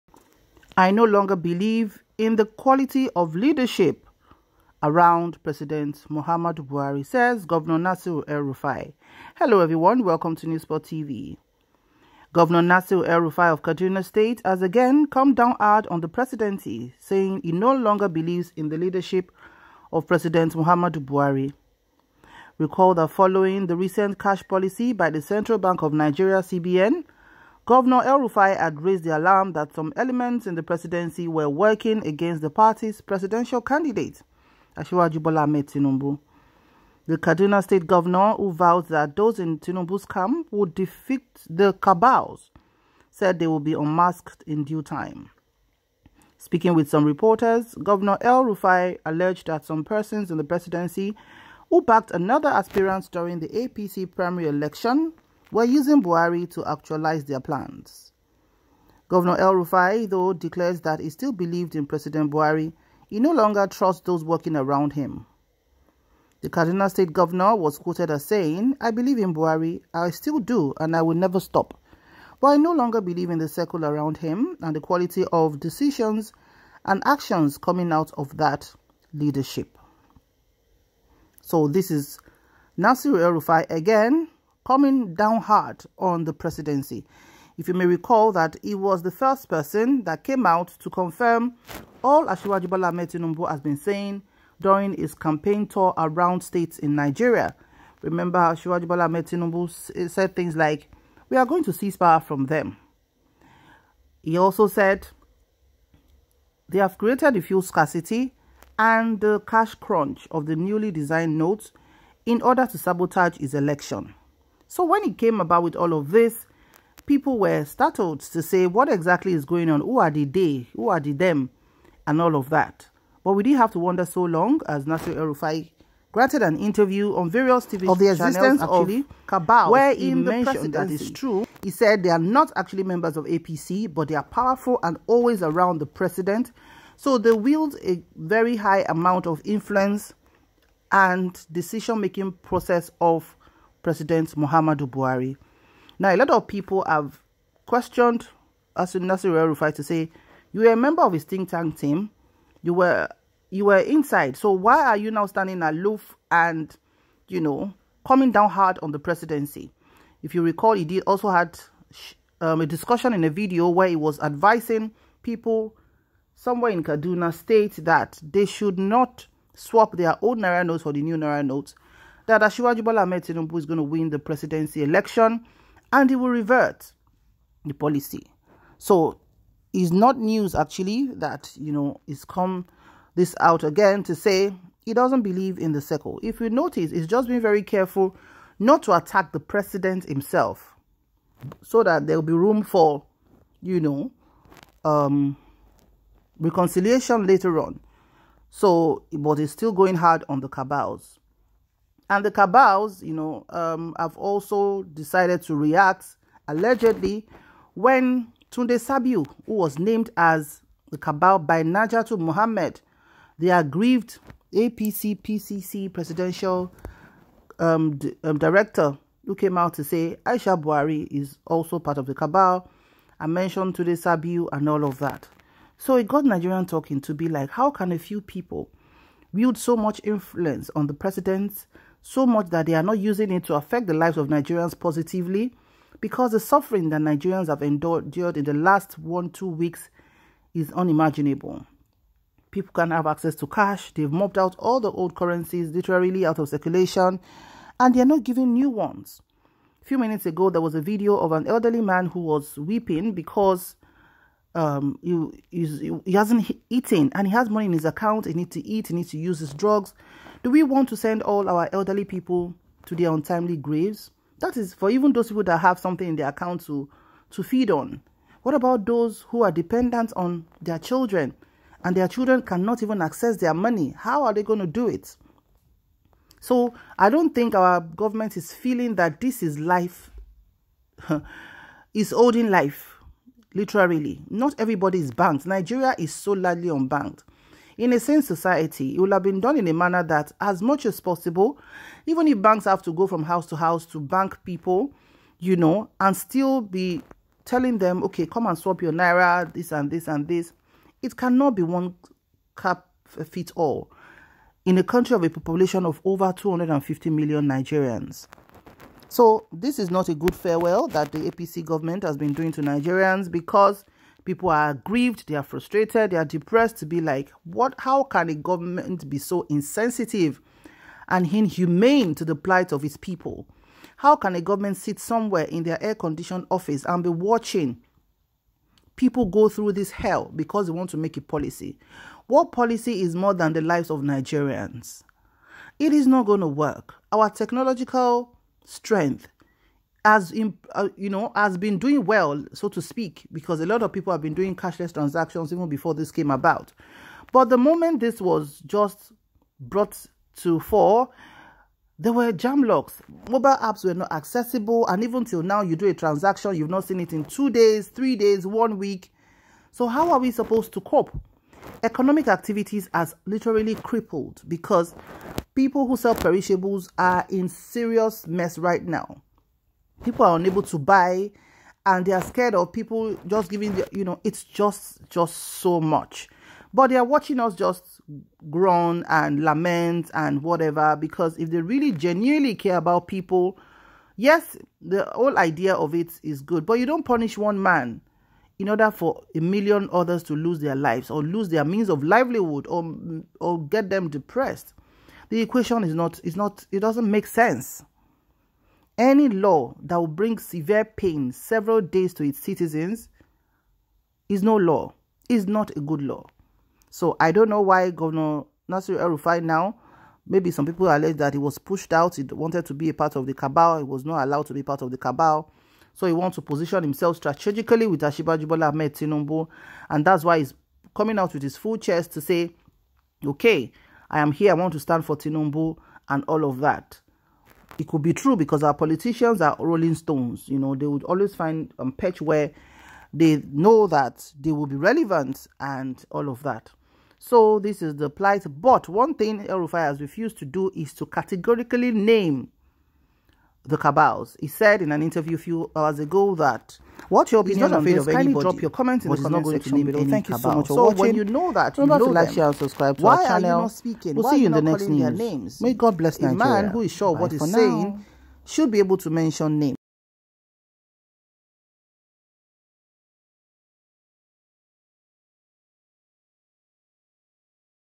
SH0CK!NG: I NO LONGER BELIEVE IN THE QUALITY OF LEADERSHIP AROUND PRESIDENT BUHARI~GOV EL RUFAI
SH0CK!NG: I NO LONGER BELIEVE IN THR QUALITY OF LEADERSHIP AROUND PRESIDENT BUHARI~GOV  EL RUFAI